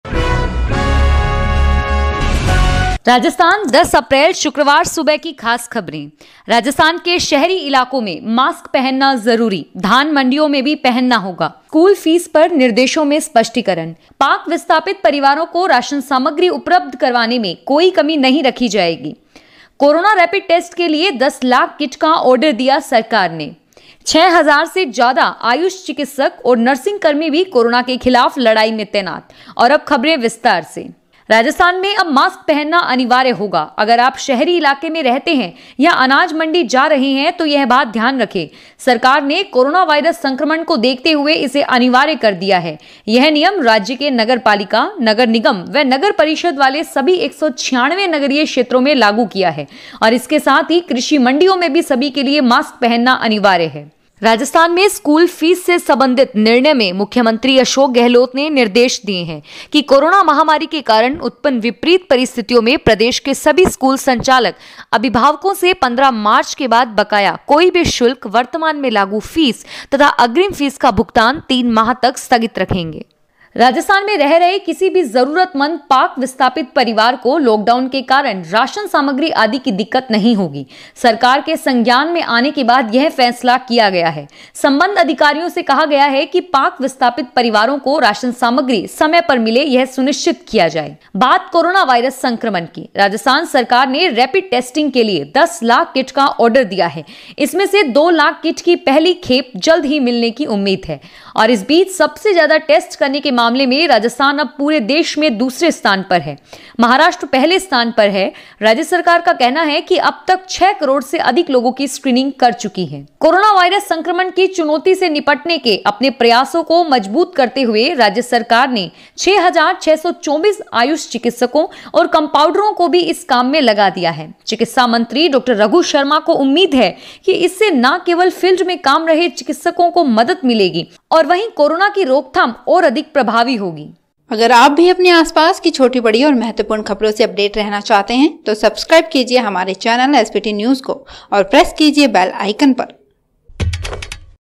राजस्थान की 5 प्रमुख खबरें : 10 अप्रेल 2020 | SBT News
- राजस्थान के शहरी इलाकों में मास्क पहनना जरूरी, धान मंडियों में भी पहनना होगा
- स्कूल फीस पर निर्देशों में स्पष्टीकरण
- पाक विस्थापित परिवारों को राशन सामग्री उपलब्ध कराने में कोई कमी नहीं रखी जाएगी
- कोरोना रेपिड टेस्ट के लिए दस लाख किट का आर्डर दिया सरकार ने
- छह हजार से ज्यादा आयुष चिकित्सक और नर्सिंगकर्मी भी कोरोना के खिलाफ लड़ाई में तैनात